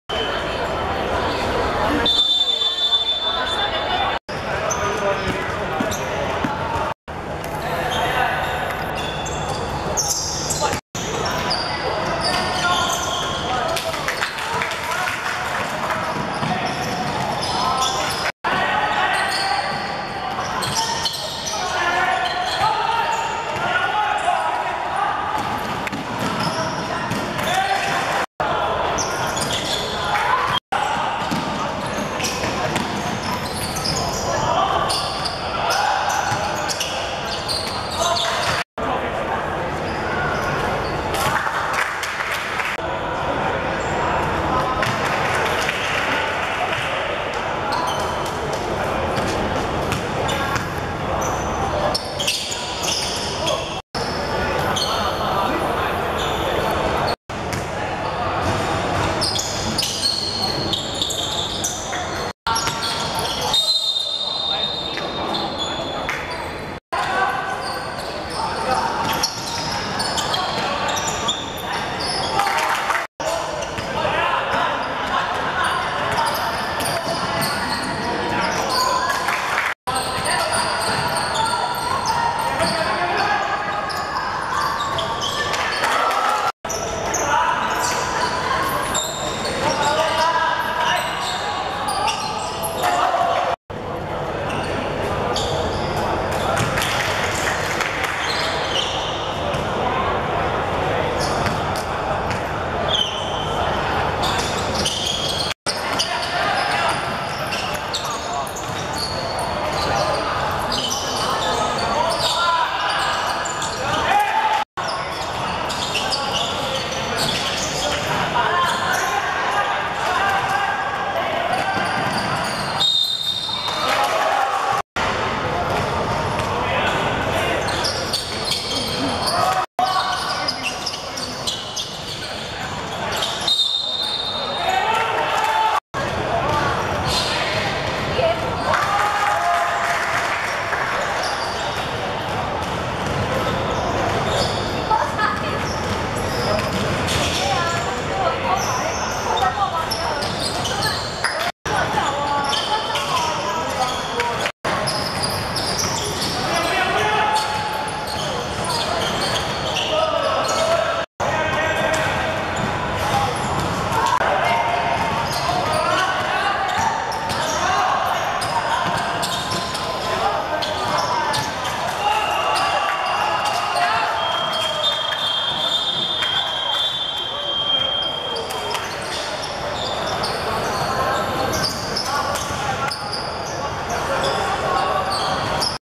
陳呂重德vs九龍聖芳濟(2015.6.25.學界籃球馬拉松男子分組賽第一圈)精華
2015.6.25.港灣道體育館 11:15 陳呂重德(藍) 19:16 九龍聖芳濟(綠)